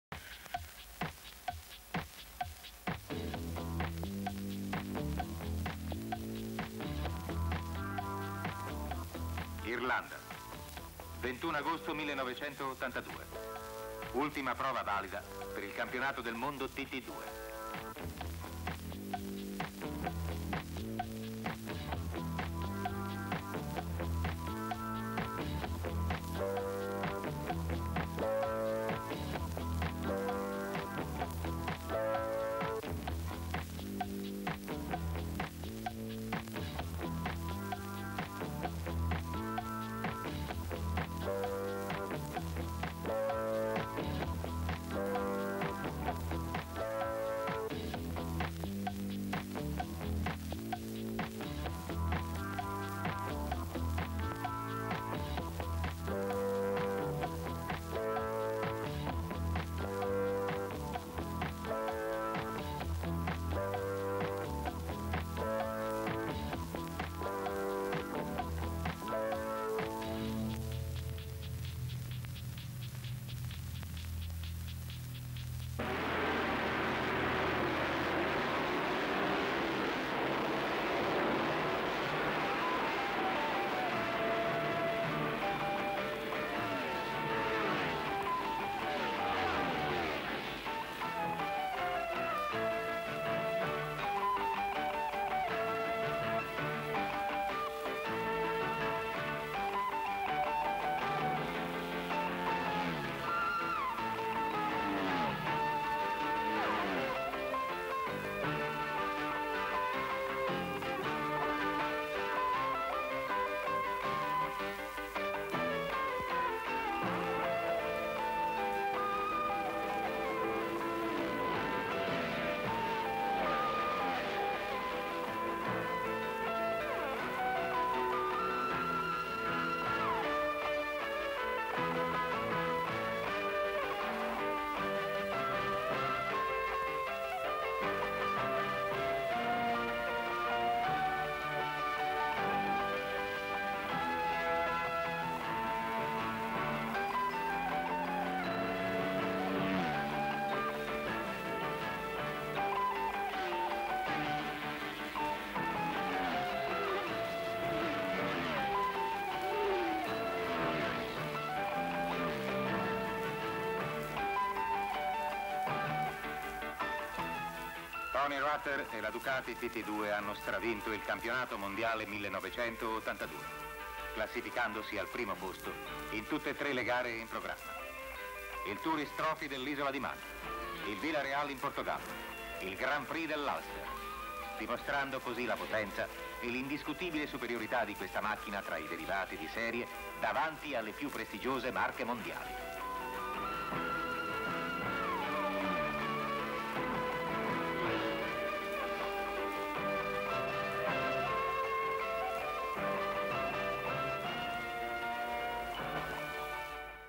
21/08/1982 UK northern ireland the TT2 race world championship at the ulster gp at dundrod
UK northern ireland motor cycling  the TT2 race world championship race at the ulster gp at  dundrod   the dundrod circuit  world TT2 championship  tourist trophy    pack of riders on the stretch   several ducati  ducati  close up tony rutter with crush helmet on the grid  panning on paddock   pit     # 7  TT2 ducati of tony rutter  racing twin based on the production pantah franco farne smoking cigarette     last round  rutter pushing # 7   fans  pit work  rain on the grid  franco farne   works # 3 1  TT2 ducati   rainer nagel of west germany  pack on the grid   a suzuki  green light   action  most with # 7   TT2 ducati of tony rutter in the lead   another # 8 TT2 ducati  standing # 7  # 14   # 8   first three rutter and two unidentified riders  riders include 1st  tony rutter ducati   donny robinson n irl yamaha  george fogarty  gb ducati    billy  hill gb honda    5th phil odlin gb  yamaha  ronan sherry    irl yamaha    7th rainer nagel d  ducati steve moynihan  gb  yamaha   john caffrey gb ducati   steve sutton gb yamaha